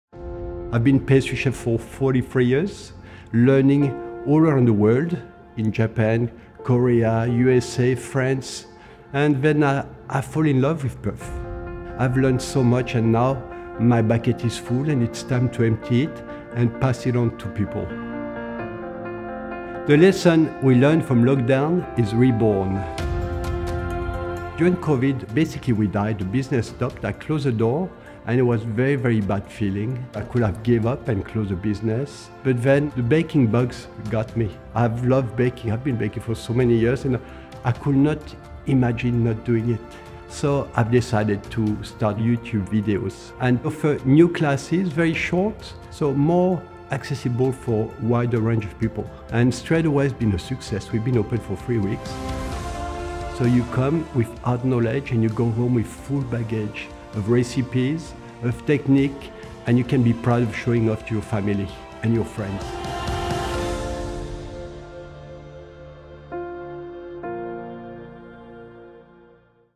Sweet Artist Academy
Reawaken your sense of adventure & discover how the experience at the Sweet Artist Academy was reborn during lockdown.